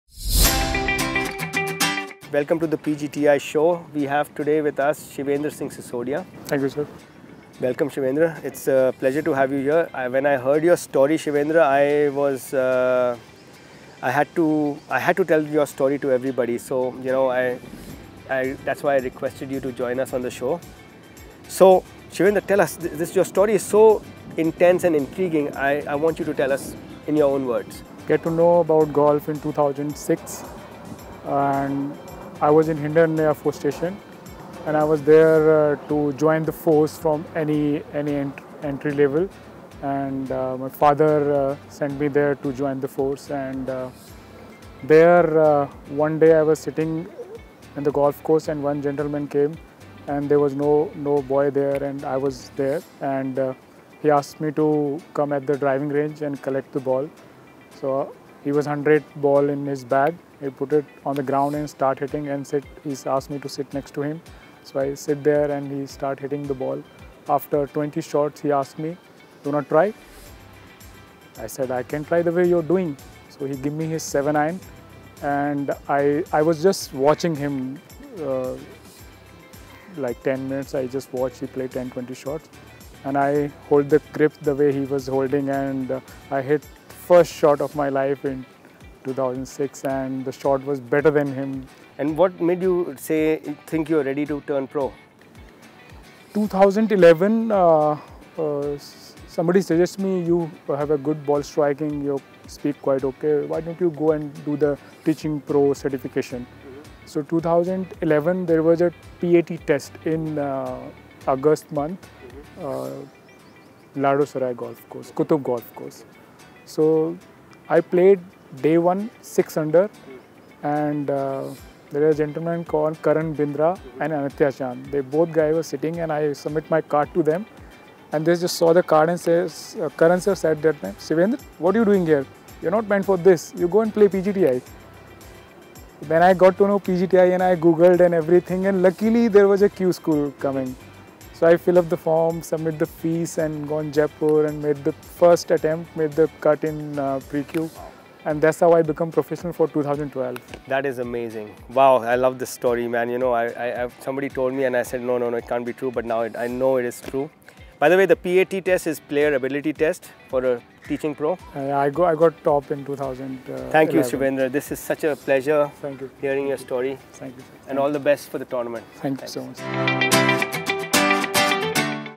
Shivendra Singh Sisodia learned golf from watching golfers play at the Hindon Air Force golf course
From the Air Force to the course! In one of the most heartwarming stories on the tour, Shivendra Singh Sisodia started his journey in golf by learning the sport from watching golfers play at the Hindon Air Force golf course, and aimed to become an instructor. Pushed by others in the Indian golf fraternity, who encouraged his natural talent, he qualified for the PGTI in 2012. Even after a decade of highs and lows, the soft-spoken golfer inspires us with his refreshing curiosity and love for the game.